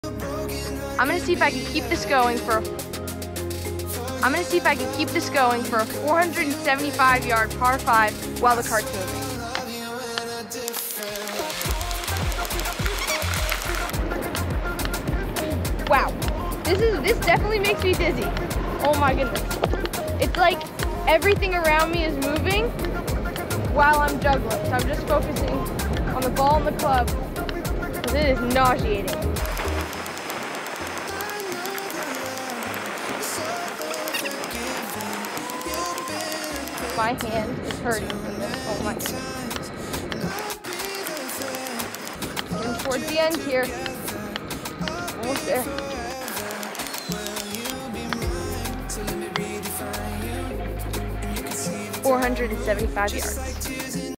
Have you ever seen anyone do this before on a golf cart?￼
I take a golf cart on a par five and juggle the ball with my golf clubs, the entire length of the hole. ￼￼